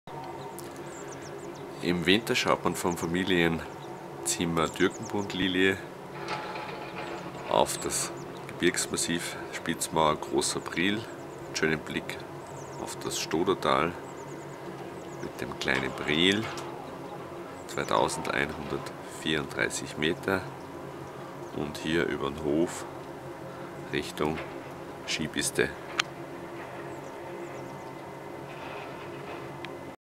Balkonblick vom Zimmer Türkenbundlilie im Winter
Dieses Video zeigt den Blick vom Balkon des Zimmers Türkenbundlilie auf die schneebedeckte Bergwelt von Hinterstoder, am Hotel Sturmgut.